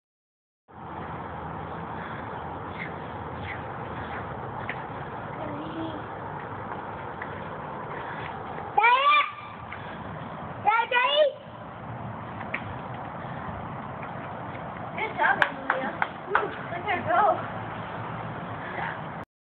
Angel Mia & mommy push up to rotation
4 year old Angel Mia Nautu decides to join the women's boot camp and does her version of push up to rotation. If she can do it, so can you!